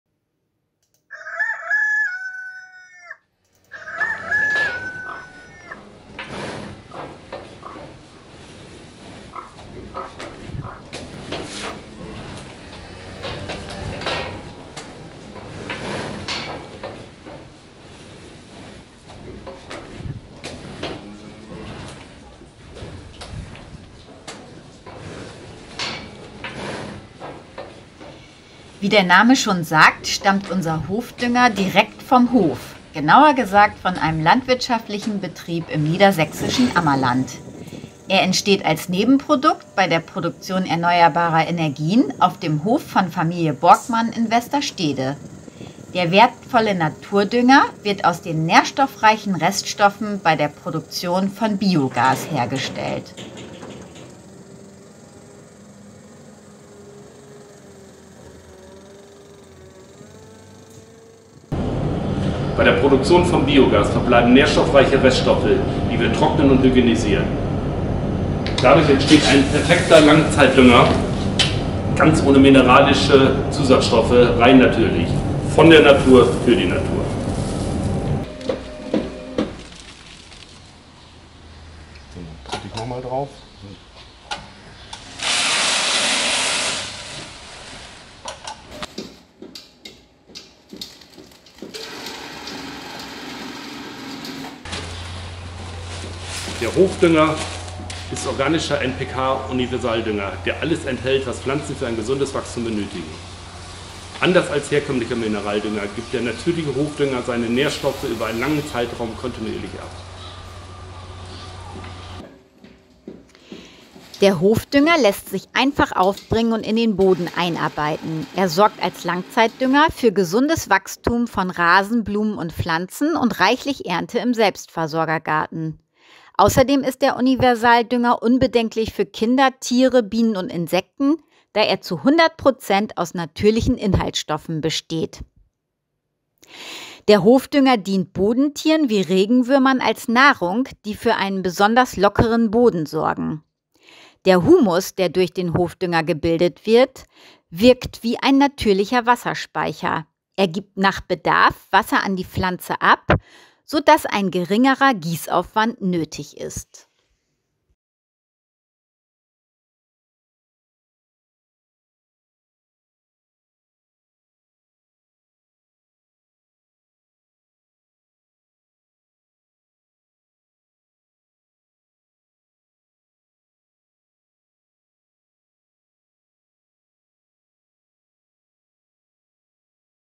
Hof Dünger_ Aus der Natur für die Natur
Unser Hof Dünger ist ein rein natürlicher Langzeitdünger - er entsteht als Nebenprodukt bei der Produktion von Biogas.